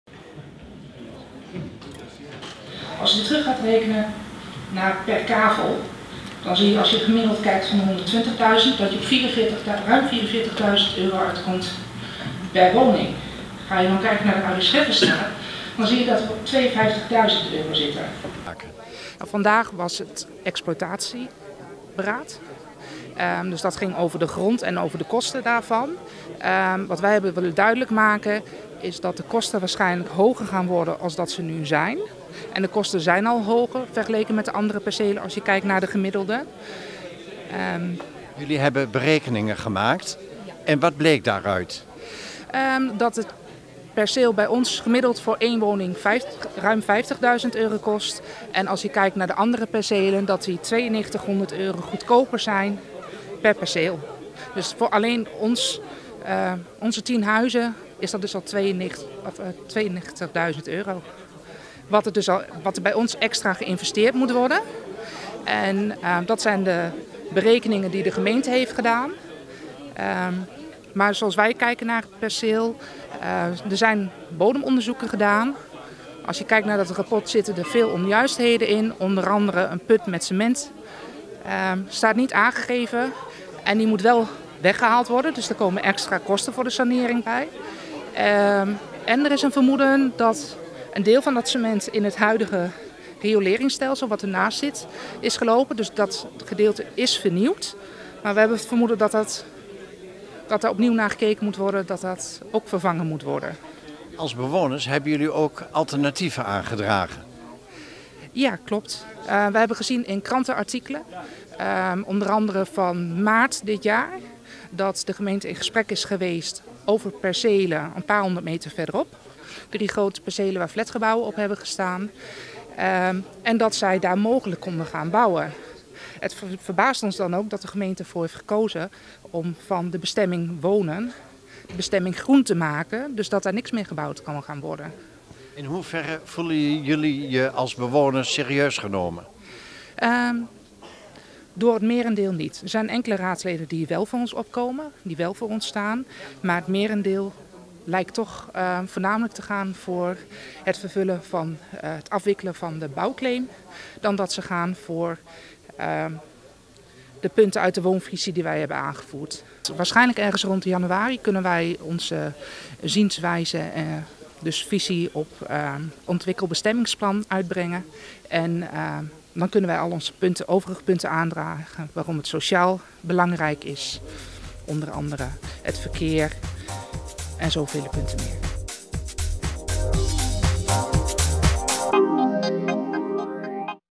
Inspraak Ary Schefferstraat
Bewoners van de Ary Schefferstraat  hebben een actiegroep opgericht en protesteren  tegen de bouw van een aantal woningen.